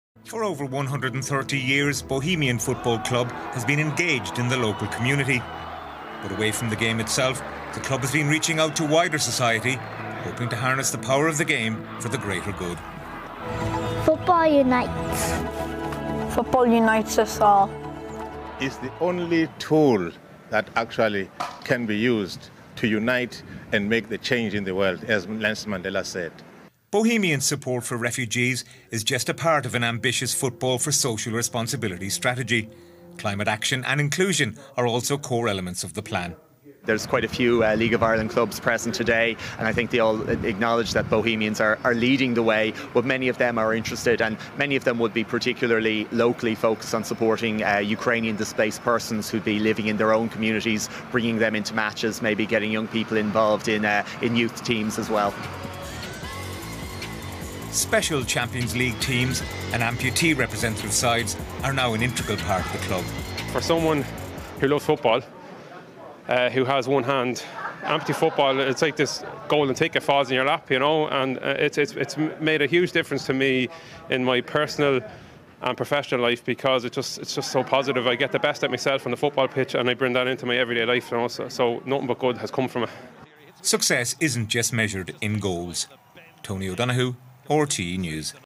Bohemian FC Football Social Responsibility/Community Strategy launched
RTÉ's report from today's launch of our five-year Football Social Responsibility/Community Strategy, which sets out how Bohemian Football Club will assist all in our communities to reach their full potential.